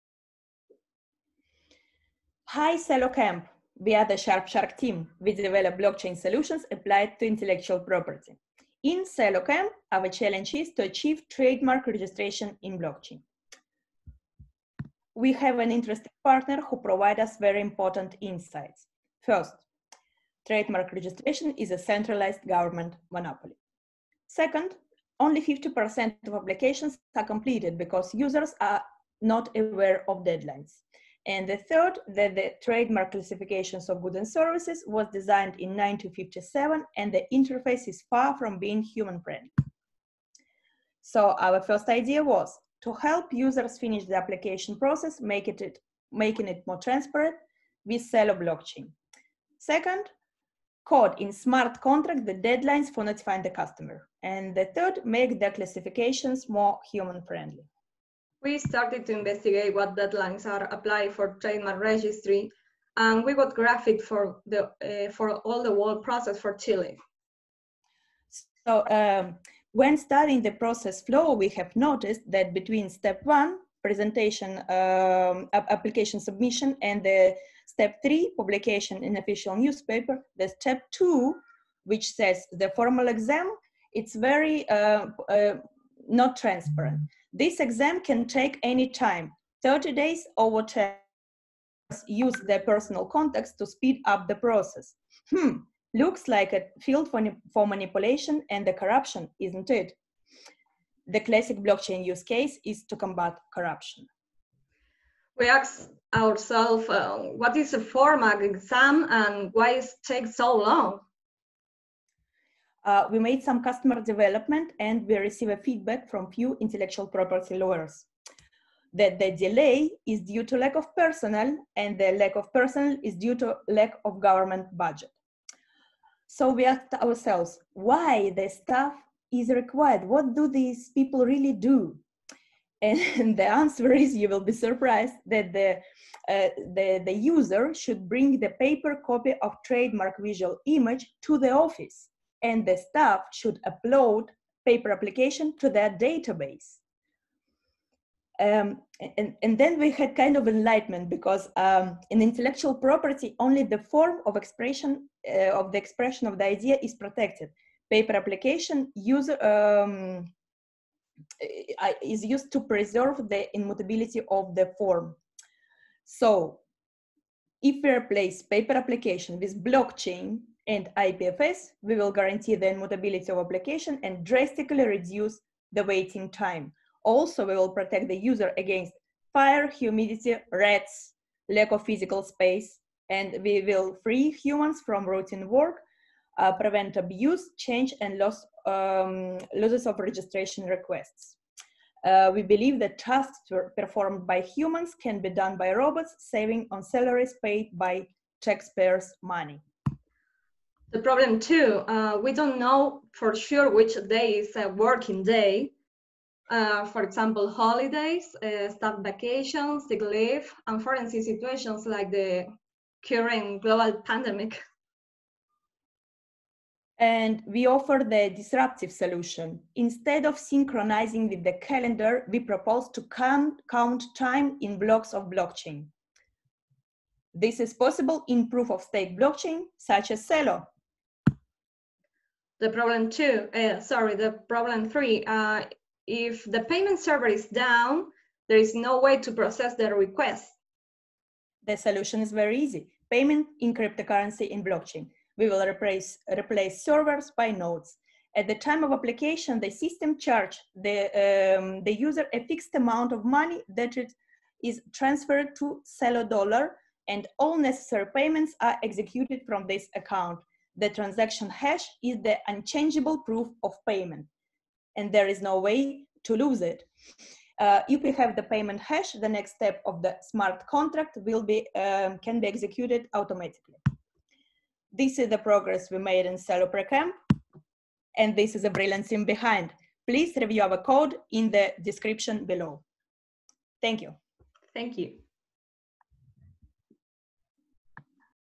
SharpShark for Celo Camp
We are the SharpShark team, we develop blockchain solutions applied to intellectual property. In Celocamp, our challenge is achieve trademark registration in Blockchain.